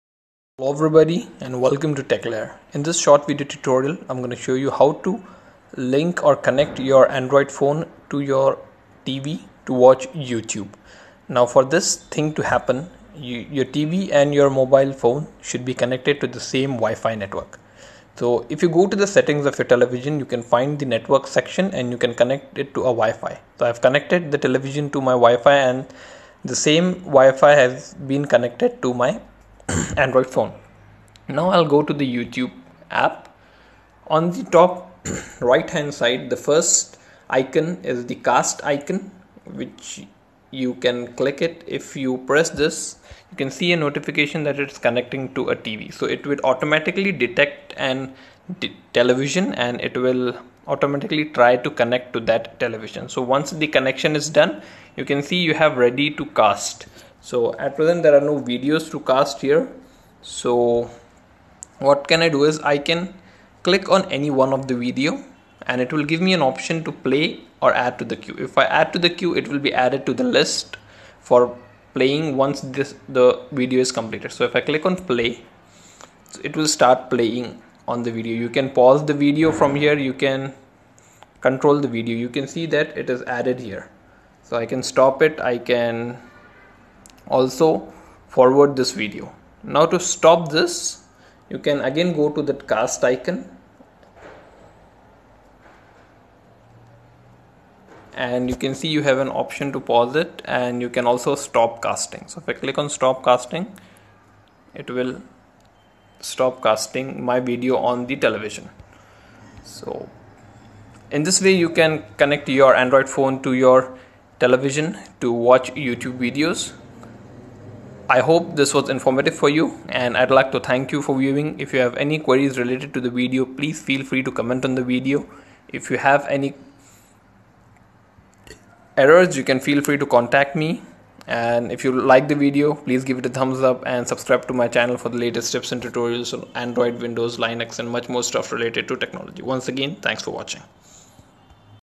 How to pair YouTube to TV from Android Phone | YouTube Help
Cast your Android YouTube App on your tv and watch it on your TV

For More Tutorials, Tips, Tricks and Hacks on Linux, Oracle, Android, Windows, Whatsapp click on the Below Links.